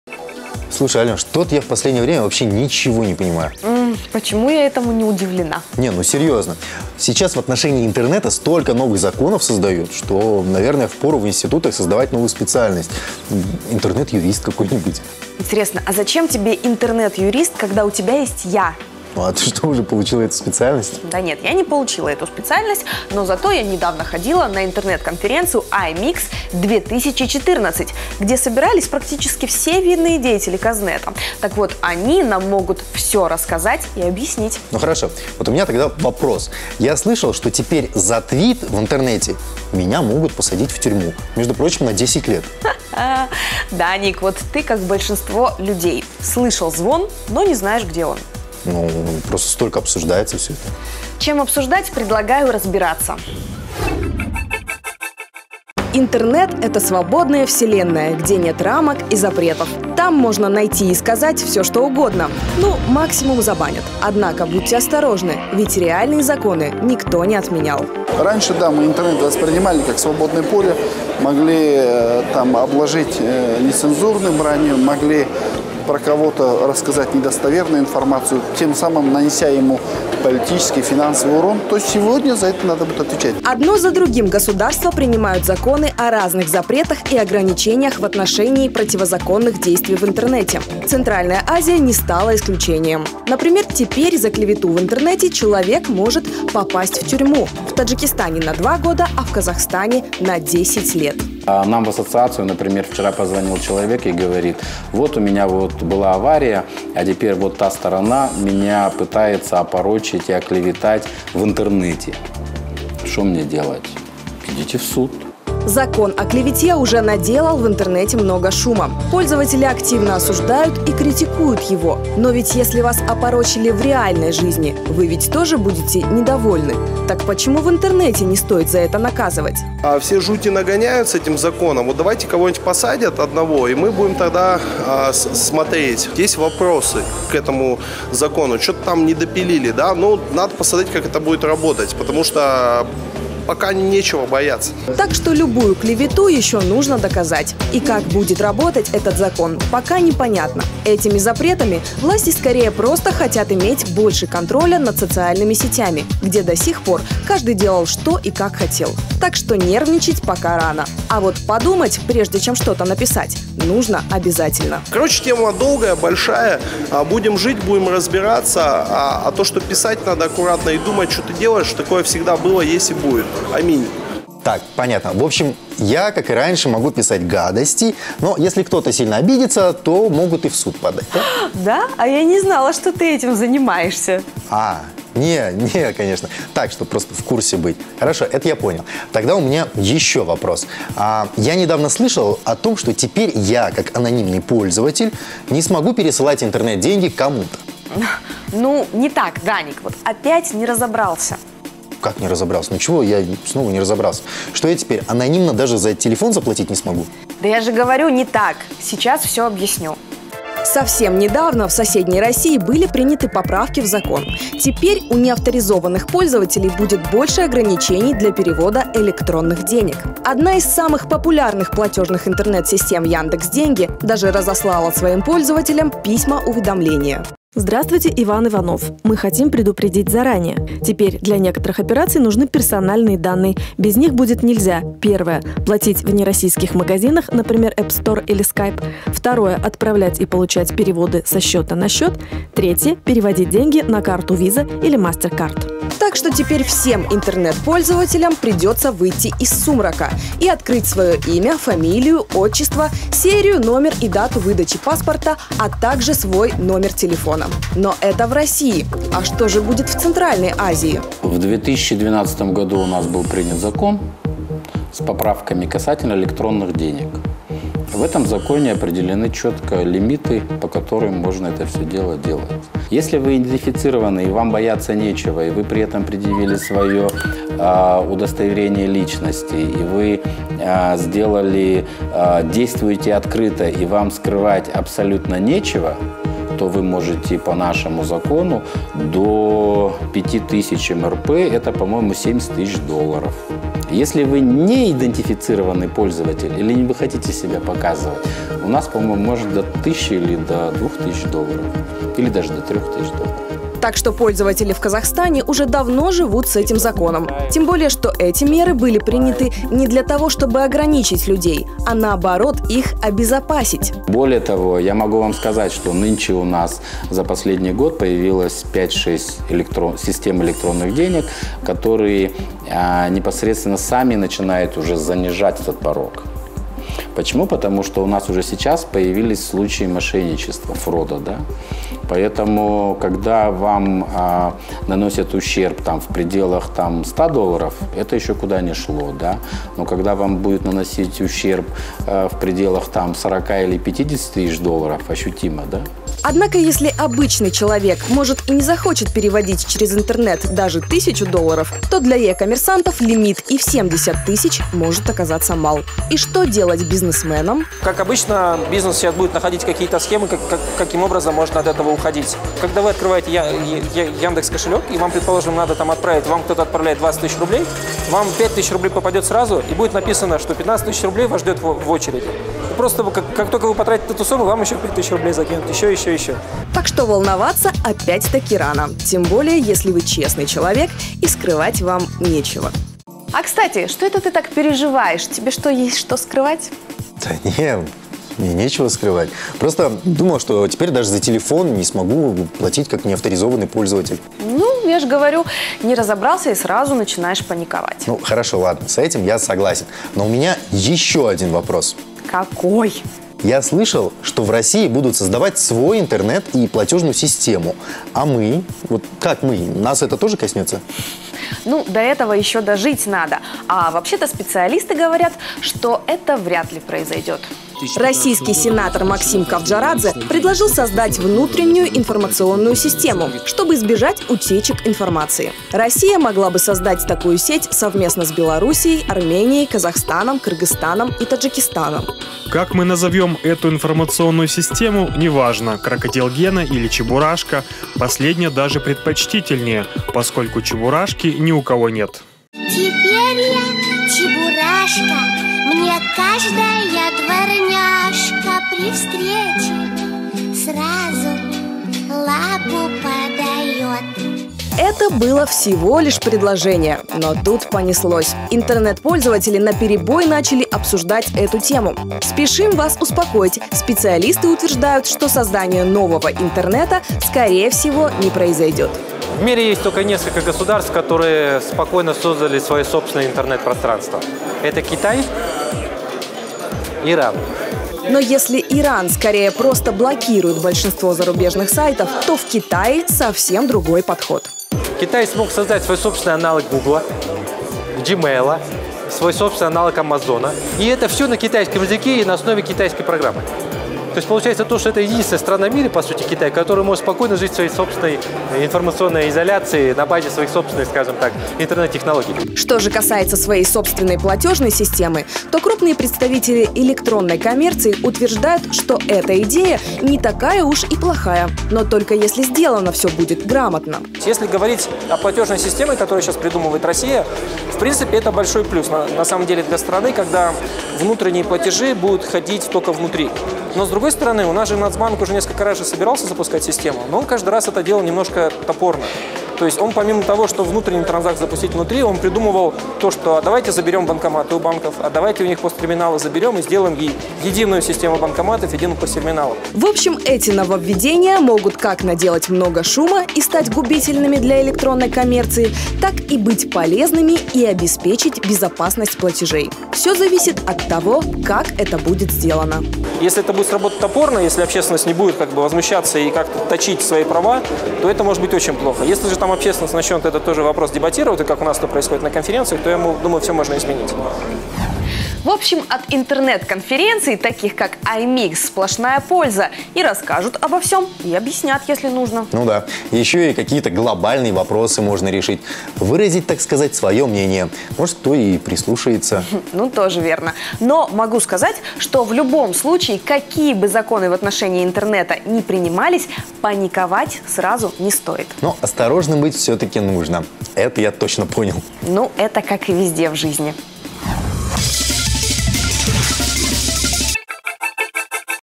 Законы в интернете: цензура или защита пользователей? (260)
В последнее время в отношении противоправных действий в Интернете появляется много законов. Также появляется много нововведений, которые диктуют нам власти. «Матрица» решила разобраться, чего ждать и, возможно, бояться странам Центральной Азии.